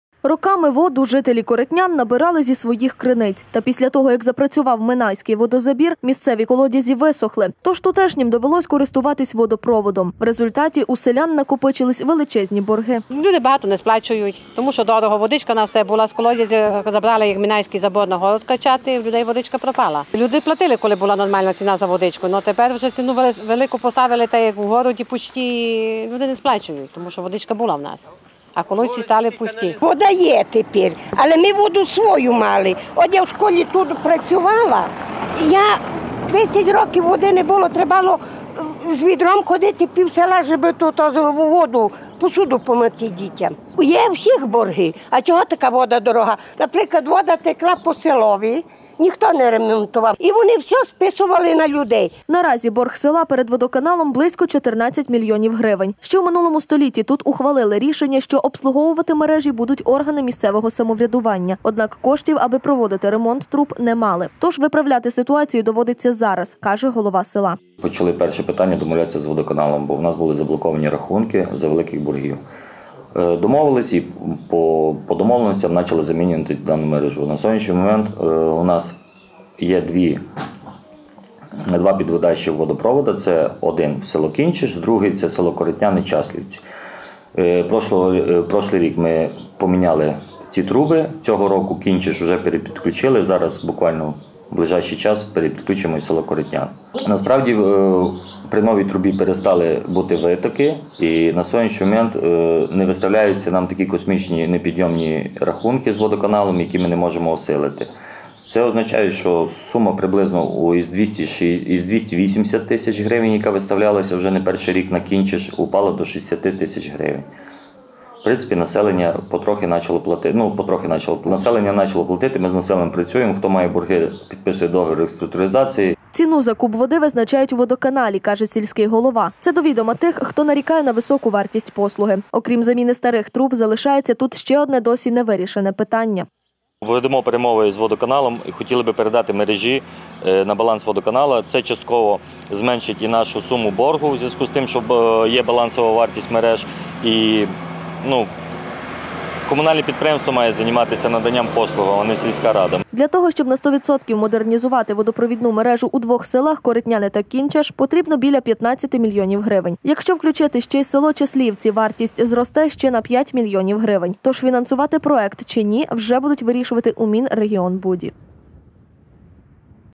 Вже з водою, але і з боргами
Досі боргують. 14 млн. грн. Таку суму повинні сплатити за використану воду жителі Ужгородського села Коритняни. Місцеві кажуть, борг накопичився через те, що вода витікала через діряві труби, а ці втрати просто розділили серед місцевого населення. Для того, щоб на 100% модернізувати водопровідну мережу у двох селах Коритняни та Кінчеш потрібно біля 15 мільйонів грн. Якщо включити ще й село Часлівці, вартість зросте ще на 5 млн. грн. Тож, фінансувати проект чи ні вже будуть вирішувати у Мінрегіонбуді.